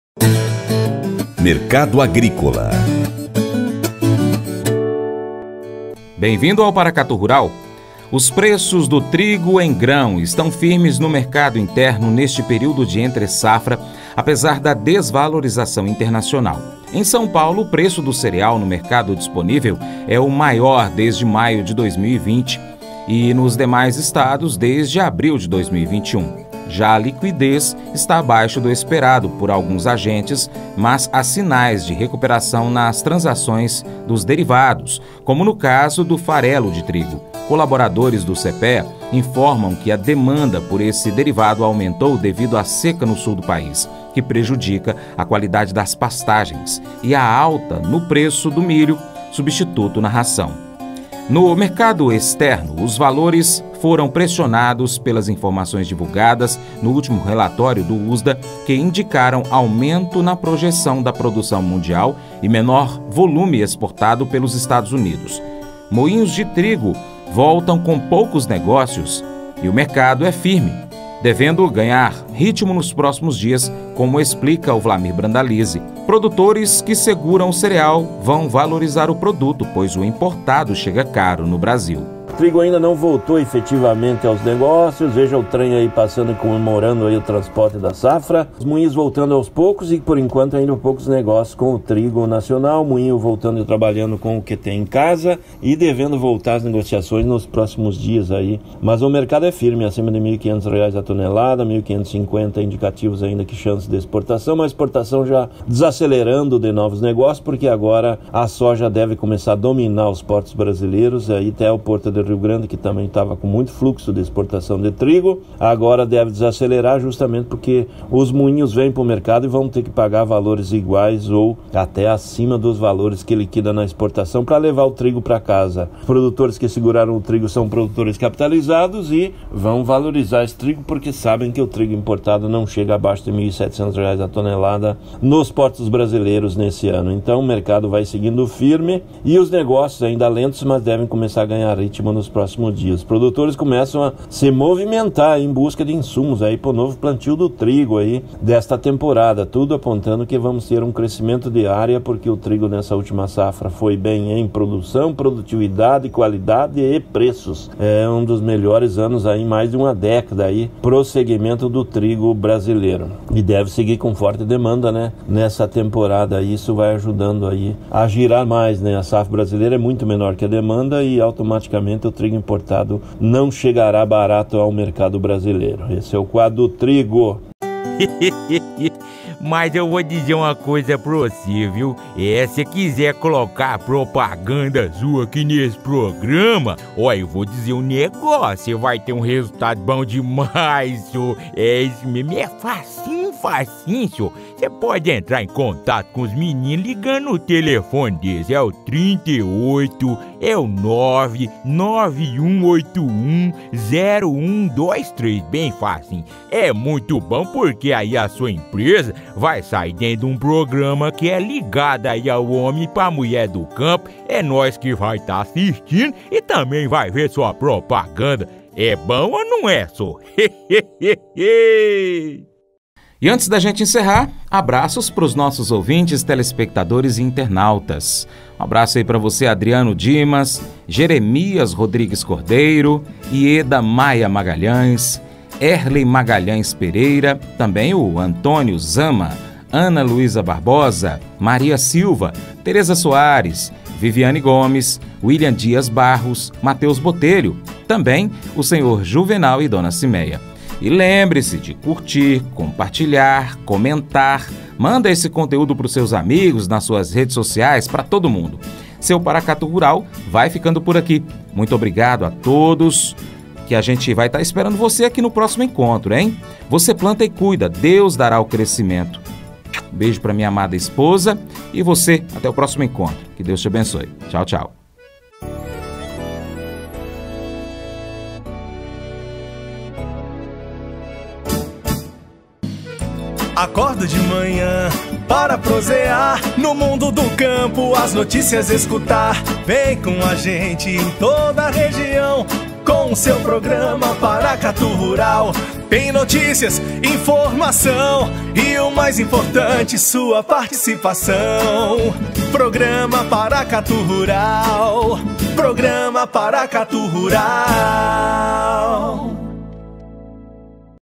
Produtores de TRIGO que seguraram cereal tem produto valorizado
Moinhos de trigo voltam com poucos negócios e o mercado é firme, devendo ganhar ritmo nos próximos dias, como explica Vlamir Brandalizze. Produtores que seguraram o cereal irão valorizar o produto, pois o importado chega caro ao Brasil.